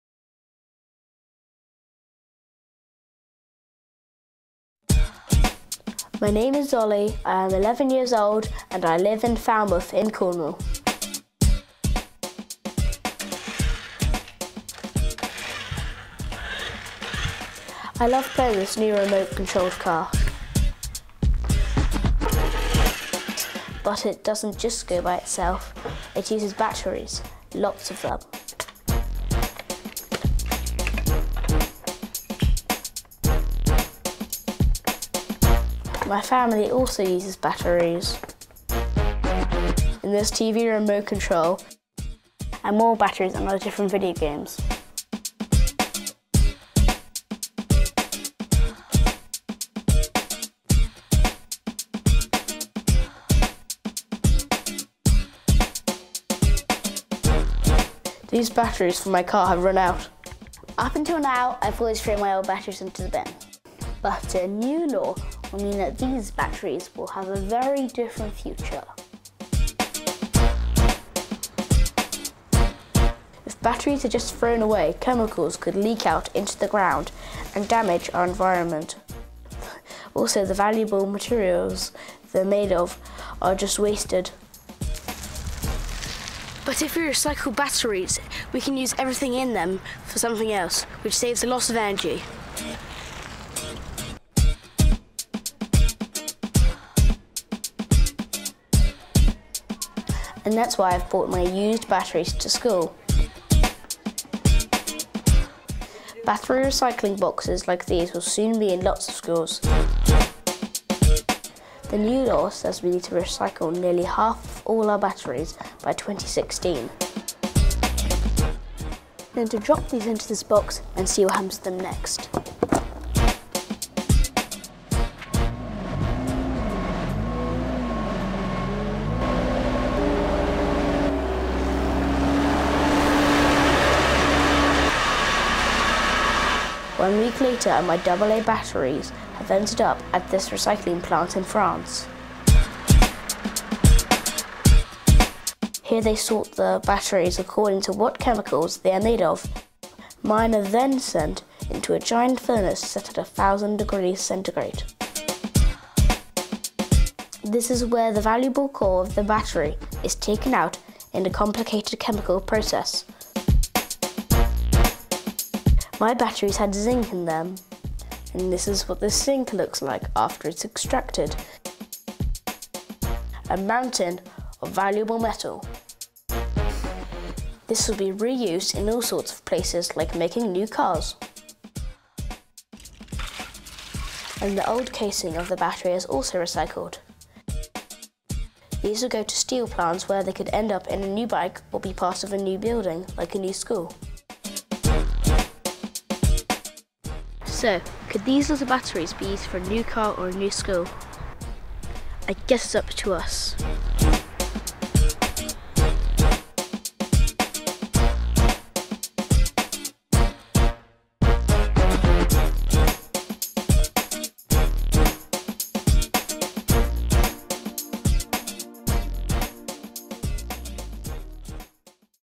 Battery Recycling Journey
Over half (58%) of 11-16 year olds have never recycled a battery. Of these, 38% didn't know that they could be recycled. Find out how batteries are recycled with this film from European Recycling Platform.